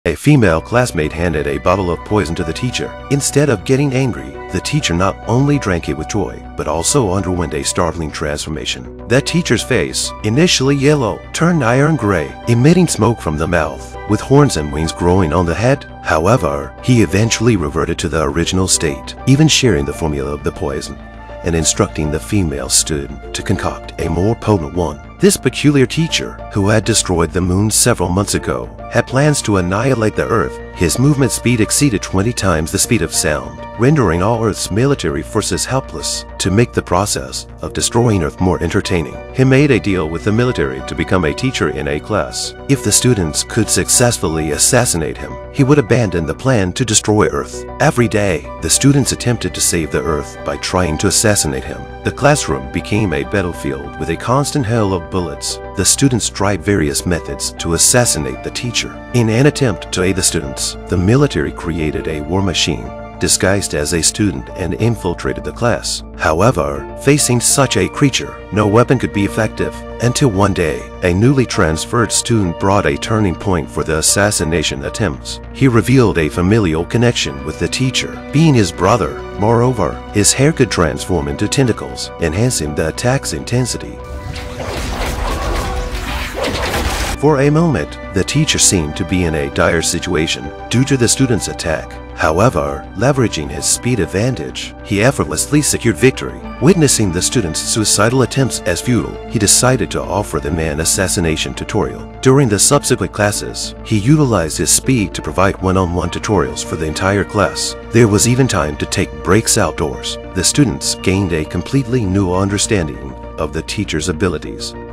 【Full Video】There's a new monster teacher at school! Finish him off! That's how you'll graduate.
Overview:There's a new monster teacher at school! Finish him off! That's how you'll graduate. not humanity
【This is Monkeys Movie~ a channel dedicated to helping you understand movie storylines better and easier, especially when they are boring and long. Can't thank you enough!】